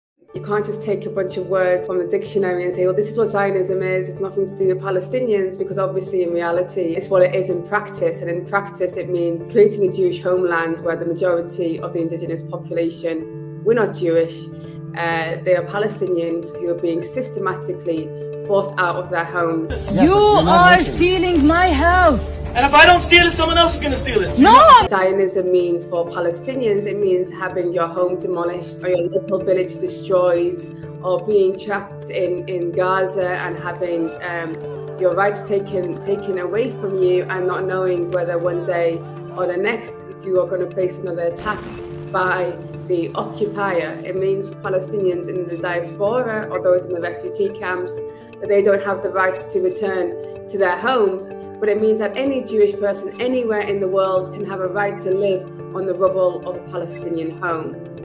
What Does ZIONISM Really Mean?
Huda Ammori explains what Zionism actually means in practice for Palestinians.

Huda Ammori is a British-Palestinian-Iraqi researcher, campaigner and co-founder of Palestine Action.

"You can't just take a bunch of words from a dictionary and say, well, this is what Zionism is. It's nothing to do with Palestinians because obviously in reality, as Ghada said, is what it is in practice. In practice it means, creating a Jewish homeland where the majority of the indigenous population we're not Jewish. They are Palestinians who are being systematically forced out of their homes. I think too Zionism for Palestinians means having your home demolished or your local village destroyed or being trapped in Gaza and having your rights taken away from you and not knowing whether one day or the next you are going to face another attack by the occupier."

"It means for Palestinians in the diaspora or those in the refugee camps don't have the right to return to their homes, but it means that any Jewish person anywhere in the world can have a right to live on the rubble of a Palestinian home."